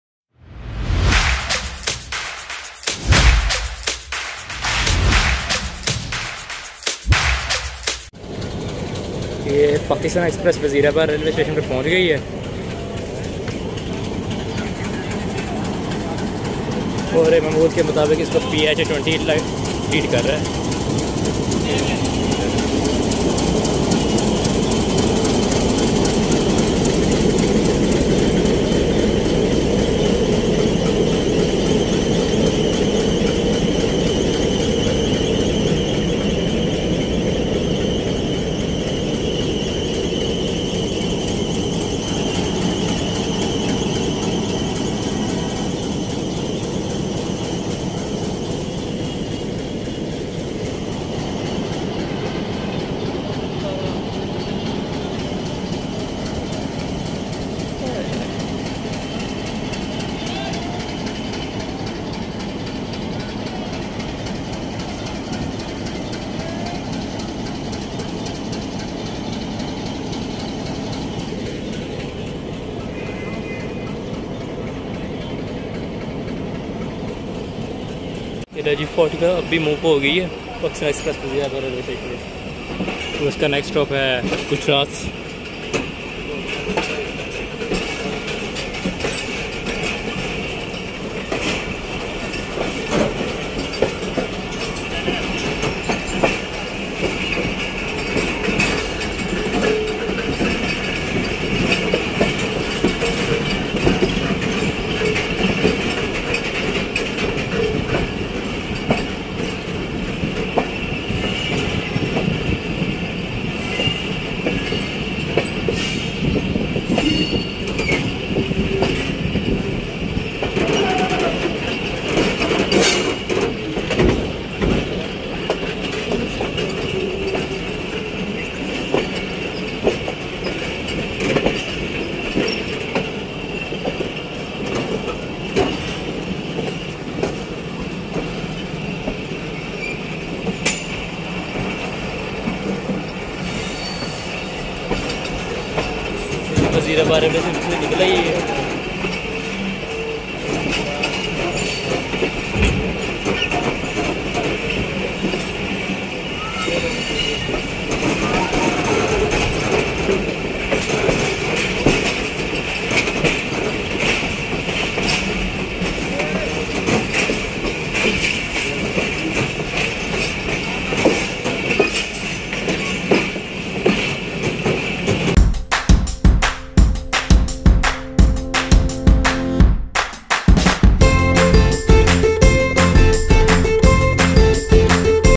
Pakistan Railways (PR): Travel Scenes of Pakistan Express Wazirabad Junction railway station.
In this video, You can see some beautiful train scenes at Wazirabad railway station. Pakistan Express (45Up) is departing from Wazirabad Junction railway station.